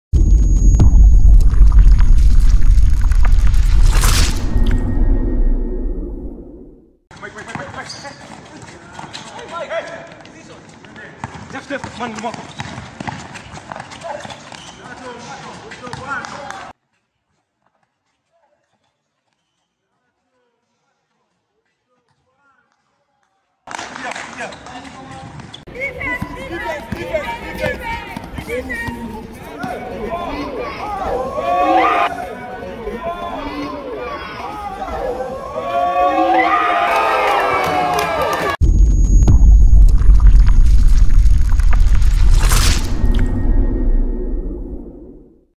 WHO DID IT BEST v2 | KSBC League | 2021 Season
comparing two almost similar plays from dunks to jelly layups to  great passes